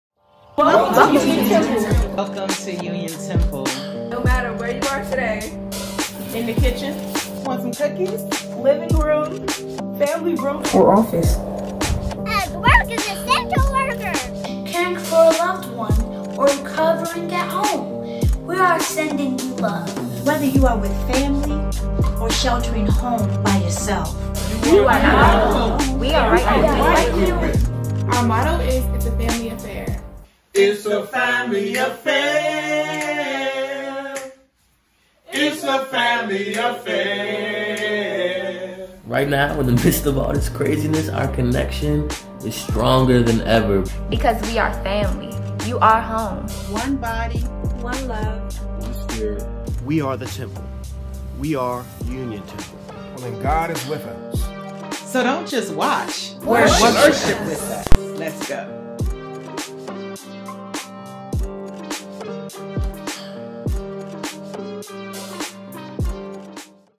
We Are Union Temple
Members of UTBC come together to remind you that we are The Temple and we're still here for one another.